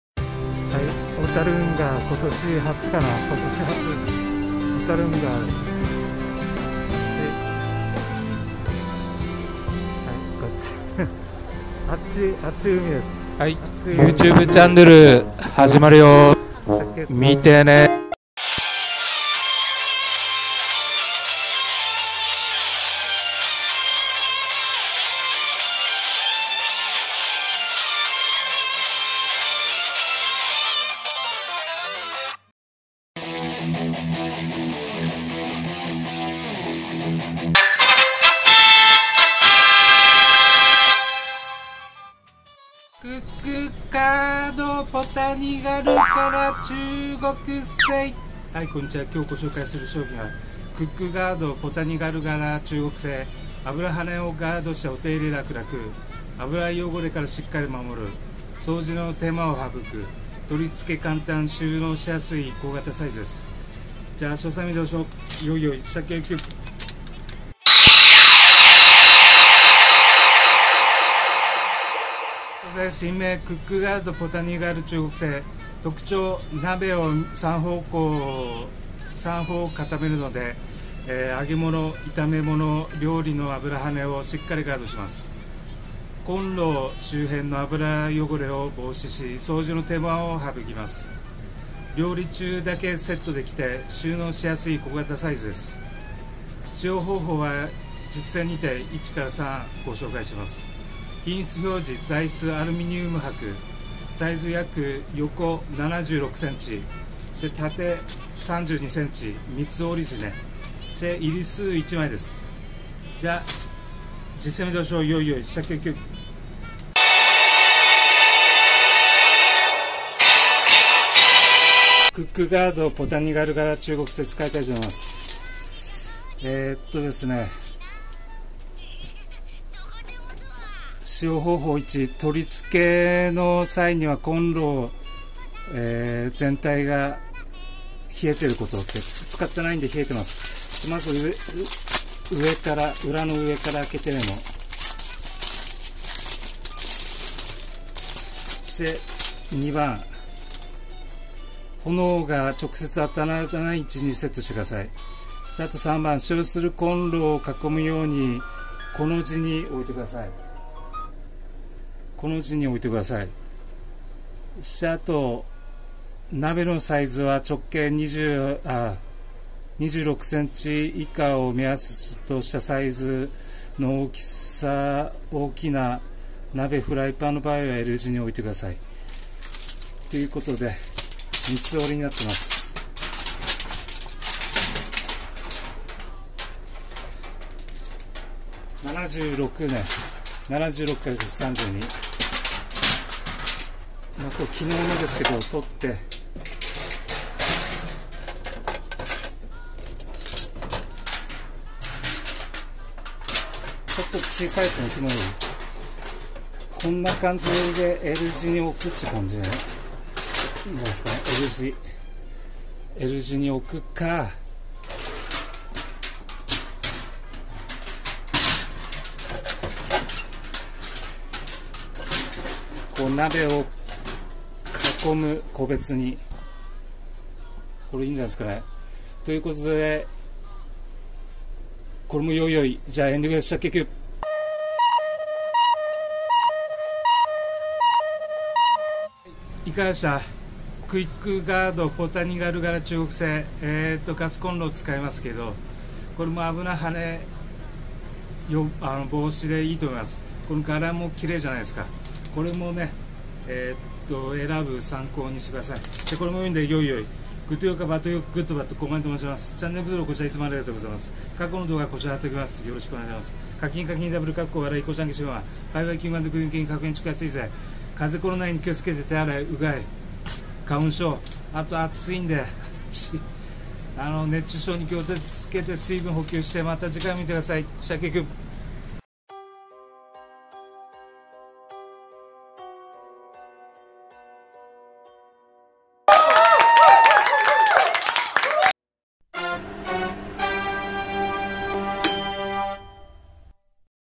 2023年7月3日@セリア商品してみた、クックガードポタニカル柄中国製品w セリア、商品紹介、100円均一、
2023年6月30日(金)
商品紹介#575
清掃用品#440
クックガードポタニカル柄中国製品
したっけキューwキュン♥

油ハネをガードしてお手入れラクラク❗️

油汚れからしっかり守る。

掃除の手間が省く。

取り付けかんたん。

収納しやすい小型サイズ。

特長

鍋を三方囲めるので、揚げ物、炒め物料理の油ハネをしっかりガードします。

コンロ周辺の油汚れを防止し、掃除の手間を省きます。

料理中だけセットできて収納しやすい小型サイズです。

✳️使用方法は実践にて①～③

品質表示

材質:アルミニウムはく

サイズ：約横76㎝×縦32㎝
(三ツ折)

入数:1枚

中国製品www